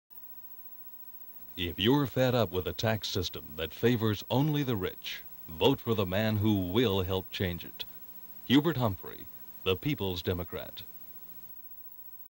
Hubert H. Humphrey [Democratic] 1972 Campaign Ad "ID"
Shot of Humphrey addressing a crowd on steps as a voice-over says if you're fed up with the tax system that favors the rich vote for a change, vote Humphrey, the people's democrat.

Creator: unknown

Location: unknown

0 minutes 12 seconds

Subjects: taxes